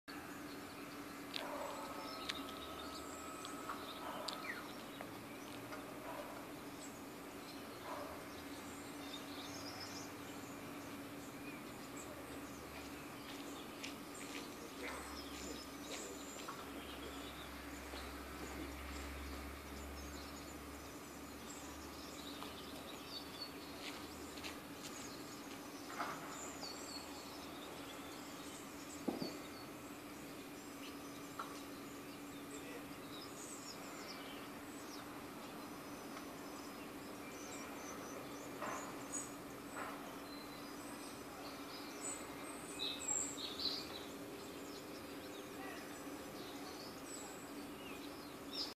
"Ape" sulla mia mano - "Bee" on my hand
Simpatico esemplare di Apis mellifera che passeggia tranquillamente sulla mia mano.
Il video è stato girato a Roma.